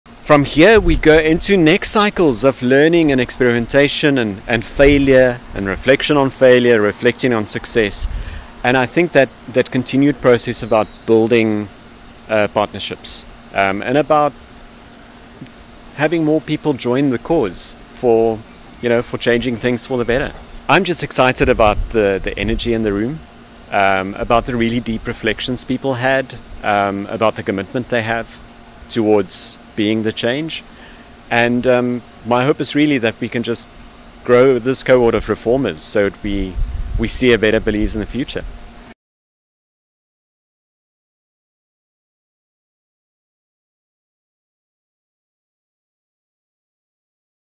Anti-Corruption Workshop: Love Foundation Empowers Account-a-preneurs in Belize
The workshop will conclude on Sunday, but Roets says there is another phase that will see additional engagements.